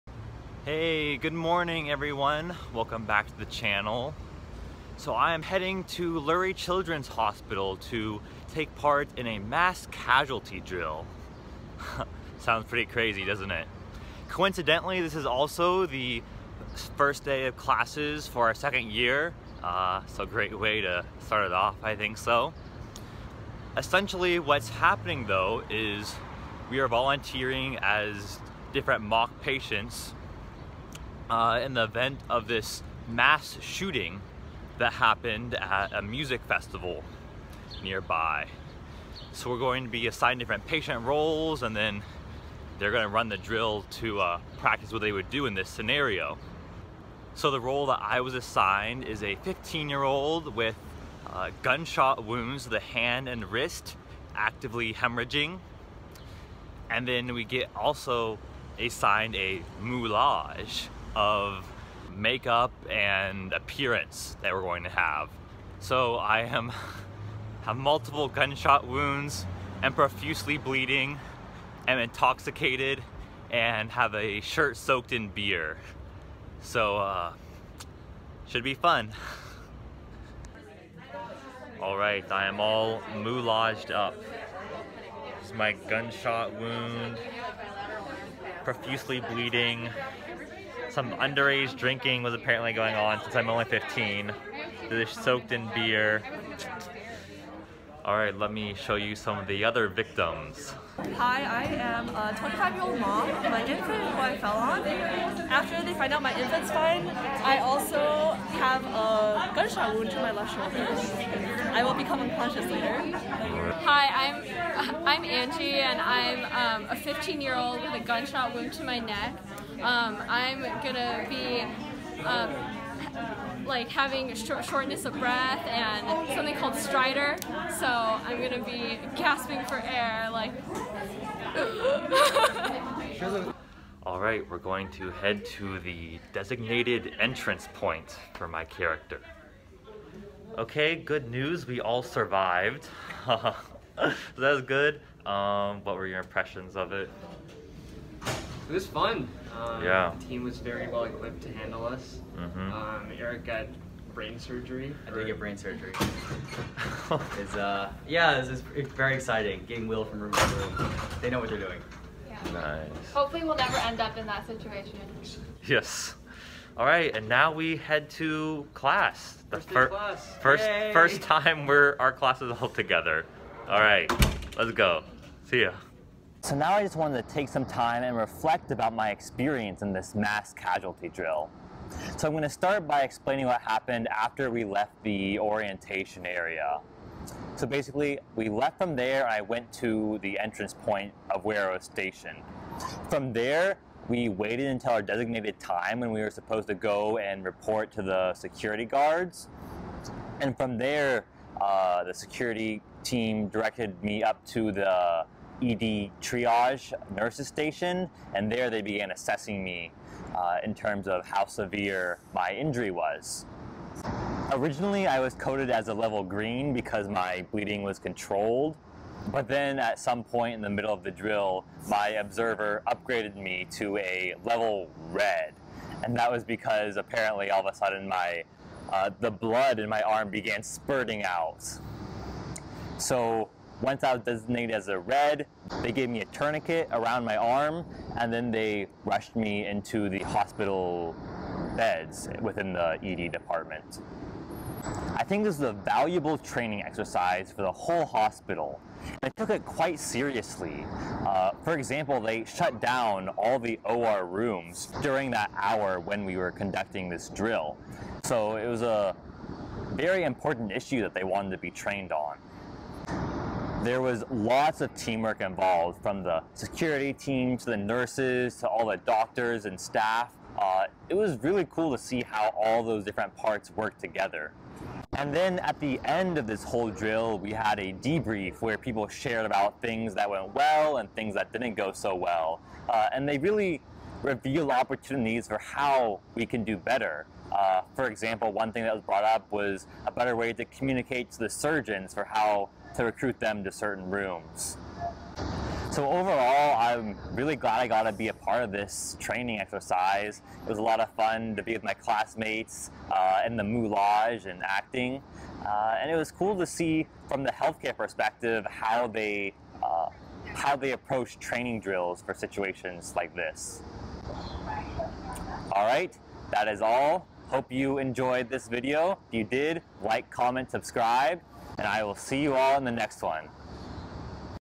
Mass Casualty Drill - VLOG
Hey everyone, in this video I'm vlogging about my experience volunteering in a mass casualty drill! Hope you enjoy!